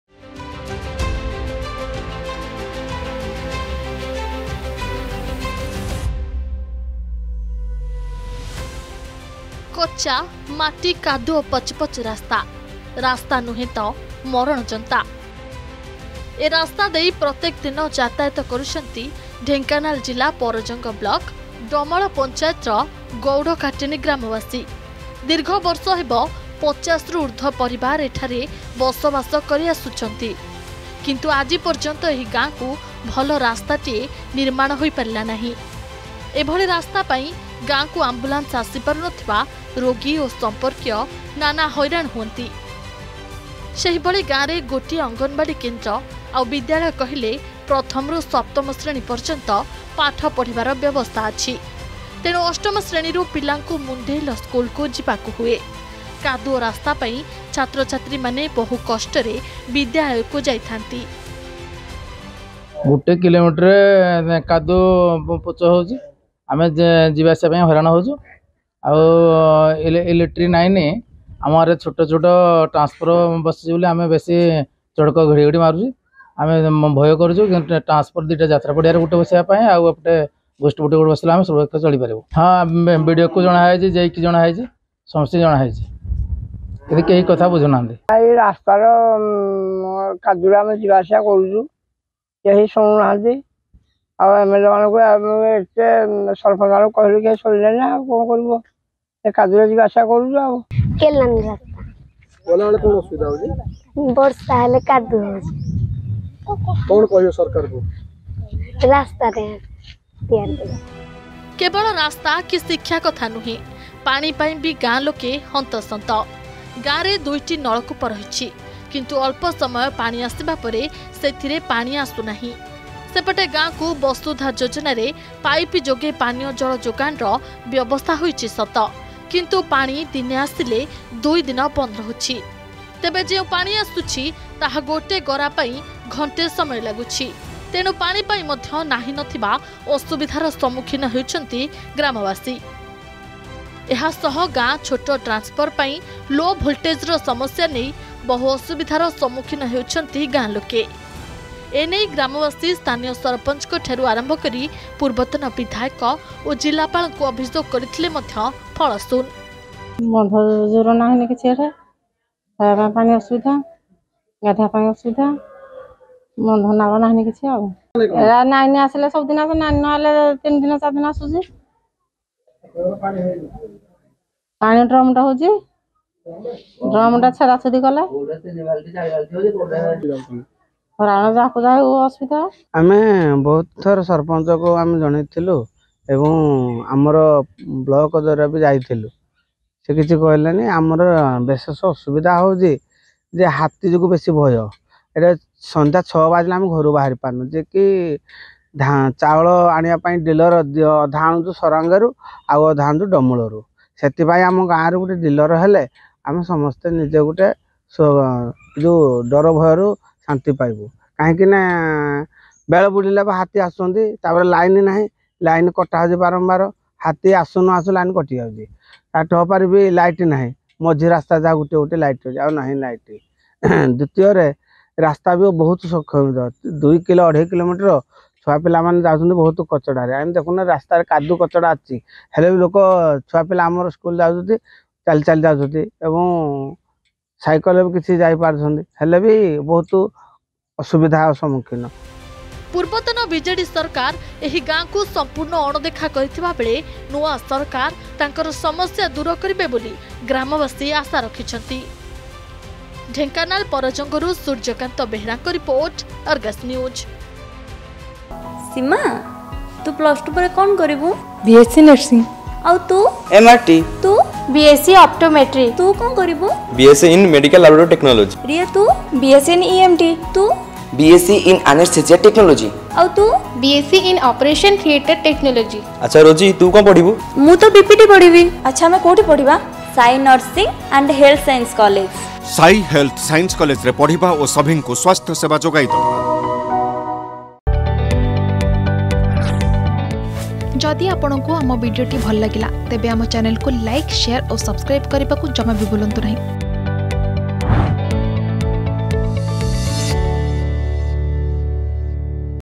ରାସ୍ତା ରାସ୍ତା ନୁହଁ ତ ମରଣ ଯନ୍ତା || Road Problem Dhenkanal
ରାଜ୍ୟରେ ୨୫ବର୍ଷ ର ସ୍ଥିର ବିଜେଡି ସରକାର ପରେ ମଧ୍ୟ ଗାଁ କୁ ନାହିଁ ରାସ୍ତା, କାଦୁଅ ପଚ ପଚ ମାଟି ରାସ୍ତା ଦେଇ ଯାତାୟତ କରୁଛନ୍ତି ଗ୍ରାମବାସୀ ।